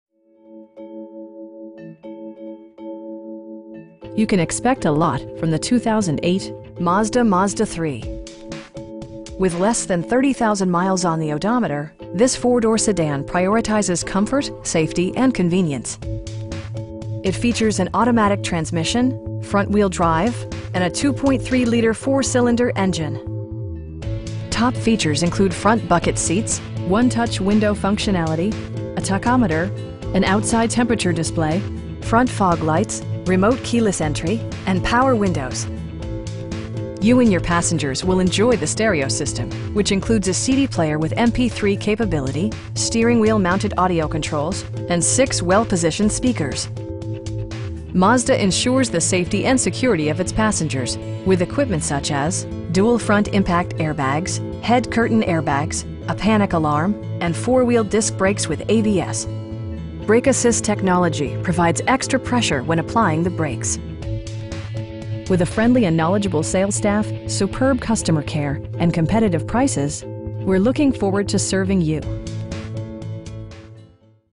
2008 Mazda Mazda3 S Touring Moonroof Alloys
Car Vision
2626 W Main St
in Norristown, PA 19403

You can expect a lot from the 2008 Mazda MAZDA3. With less than 30,000 miles on the odometer, this 4 door sedan prioritizes comfort, safety and convenience. It features an automatic transmission, front-wheel drive, and a 2.3 liter 4 cylinder engine. Top features include front bucket seats, 1-touch window functionality, a tachometer, an outside temperature display, front fog lights, remote keyless entry, cruise control, and power windows. You and your passengers will enjoy the stereo system, which includes a CD player with MP3 capability, steering wheel mounted audio controls, and 6 well positioned speakers. Mazda ensures the safety and security of its passengers with equipment such as: dual front impact airbags, head curtain airbags, a panic alarm, and 4 wheel disc brakes with ABS. Brake assist technology provides extra pressure when applying the brakes. With a friendly and knowledgeable sales staff, superb customer care, and competitive prices, we're looking forward to serving you.